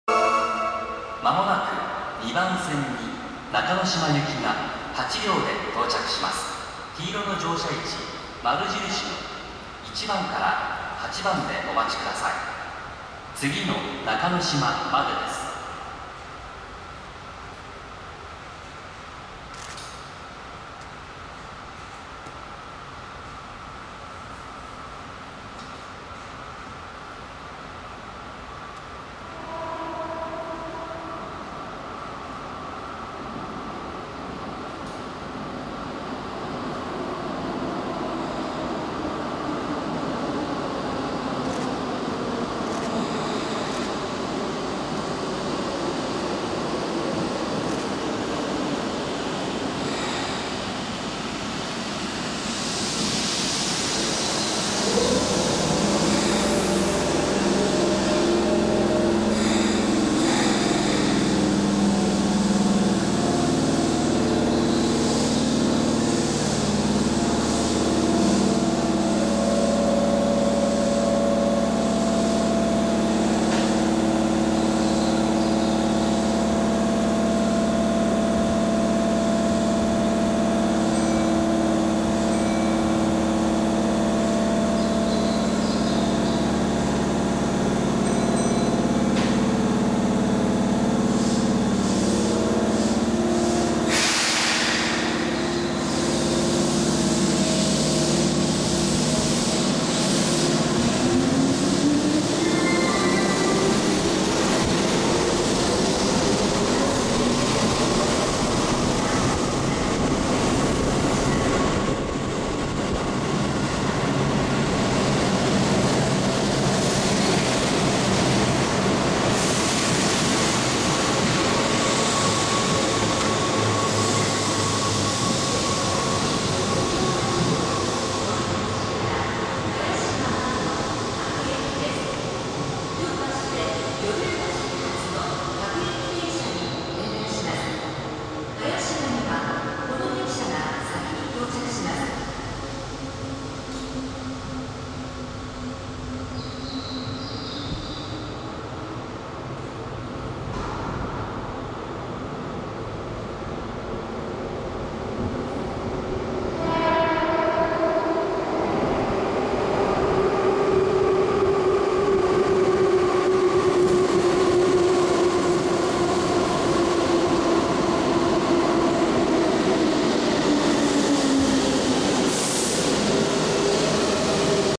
京阪中之島線・京阪本線 京阪8030系（8531F・元3000系） 区間急行中之島行き
2012/9/21撮影 渡辺橋駅、接近放送・入線・発車ベル・発車